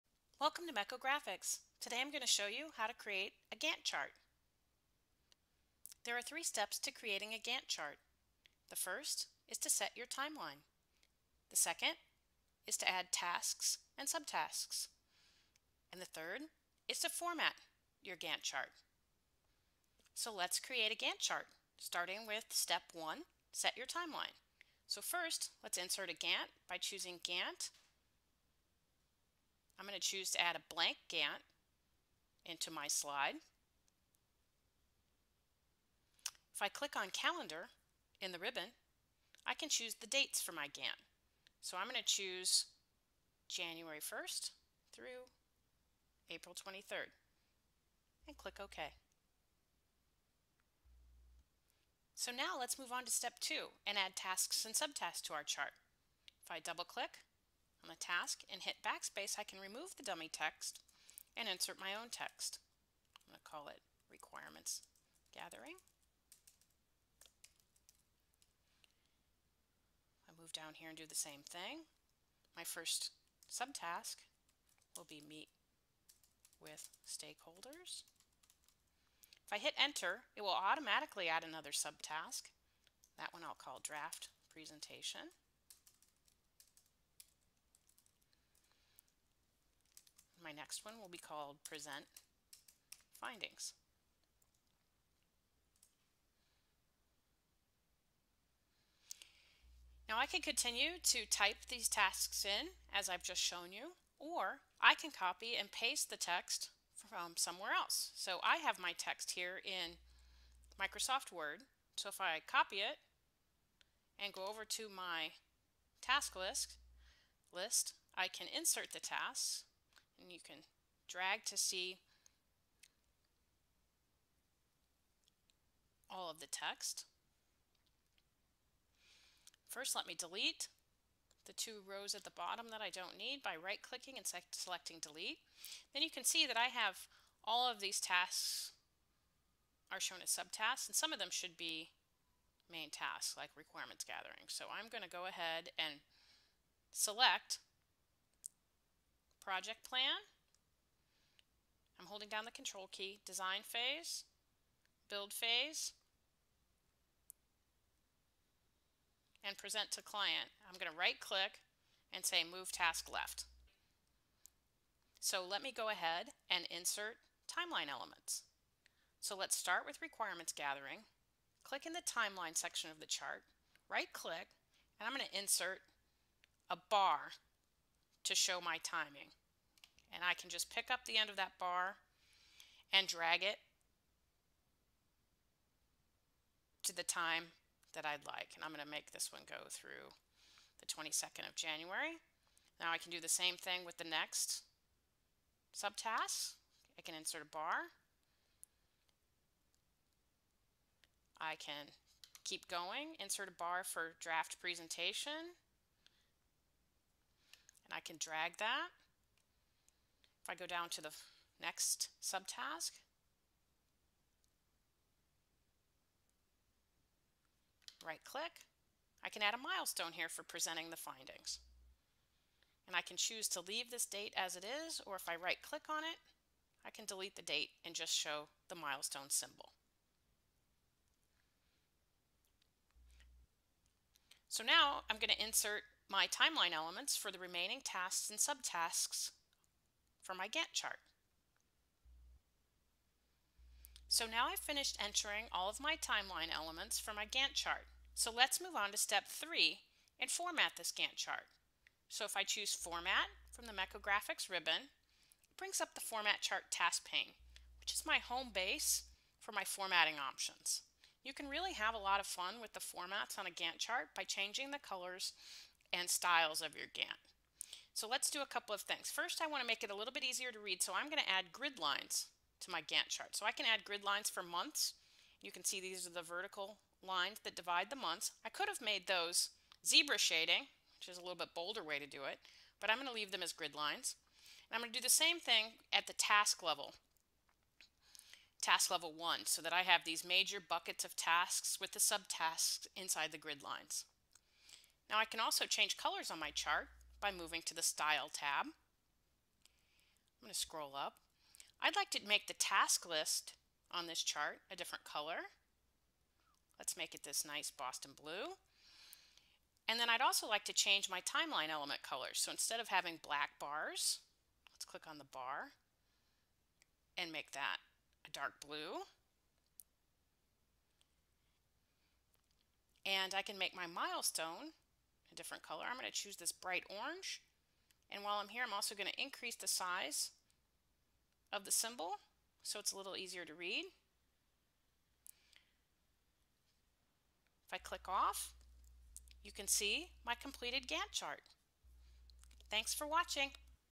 How to Create a Gantt Chart
In this video, we will show you step-by-step instructions for creating a Gantt chart using the latest version of Mekko Graphics for Windows.